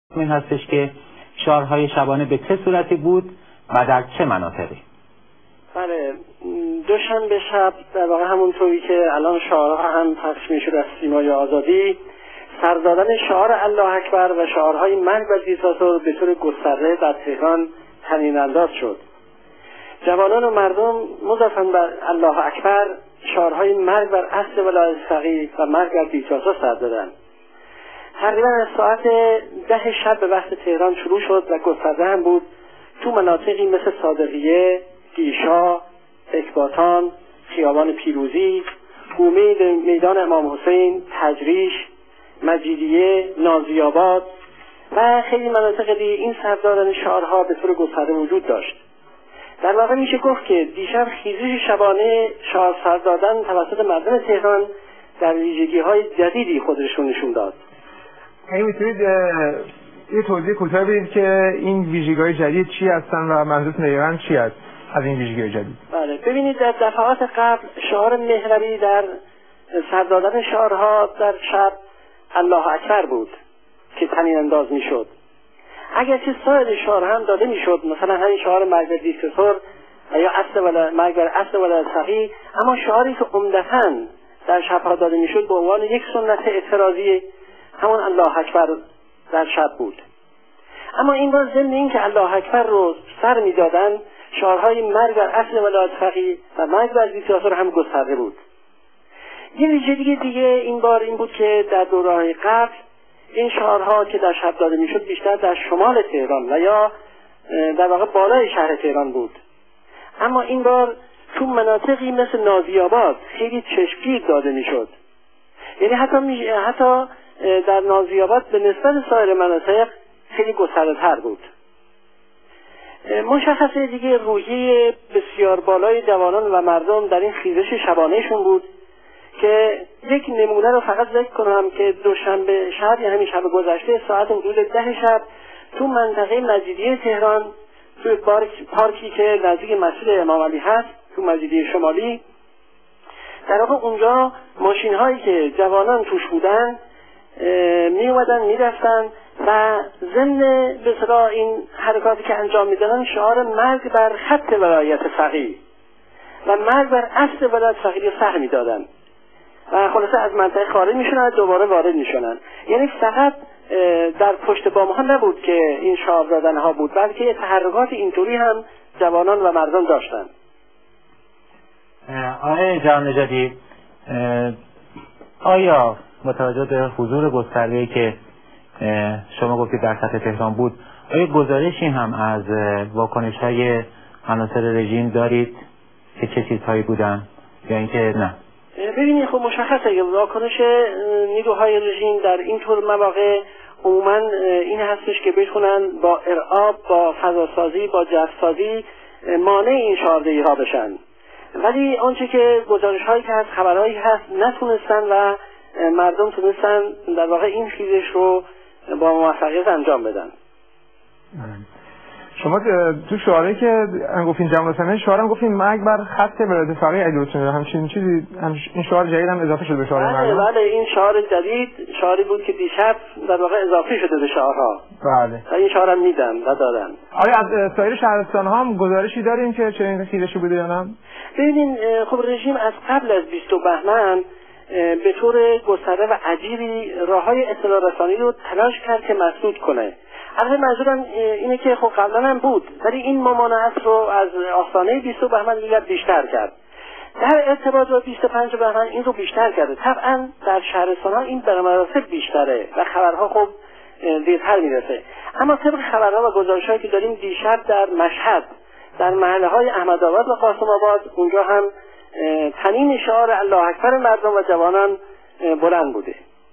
خيزش شبانه الله اكبرمرگ براصل ولايت فقيه وديكتاتور
شعارهاي شبانه دوشنبه شب الله اكبر , مرگ برديكتاتور ومرگ براصل ولايت فقيه تقريبا از 10 شب در مناطقي مثل صادقيه ,گيشا ,اكباتان ,خيابان پيروزي.
سيماي آزادي 25 بهمن90